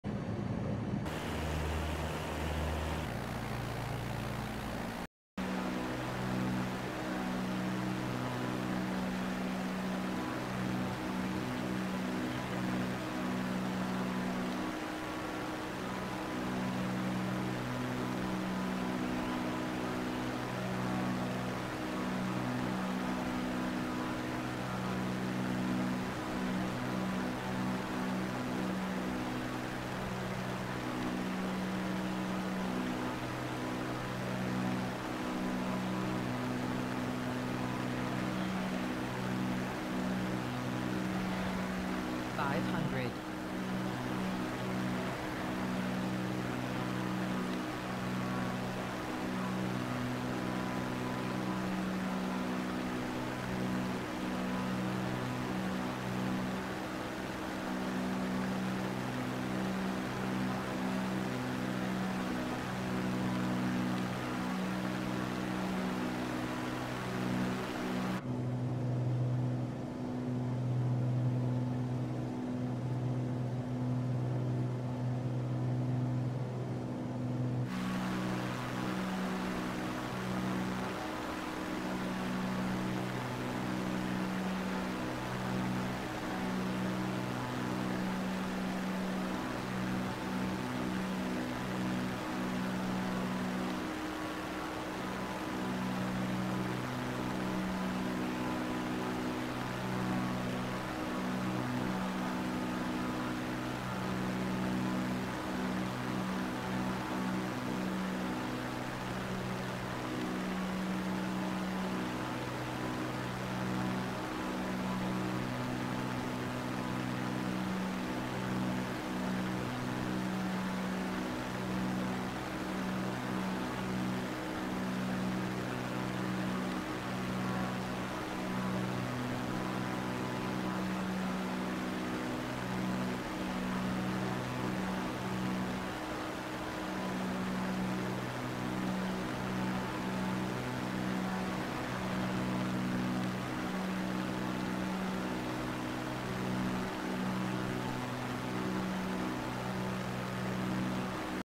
ダムフライト～新冠ダム
今回は、北海道新冠郡新冠町にある新冠ダムです。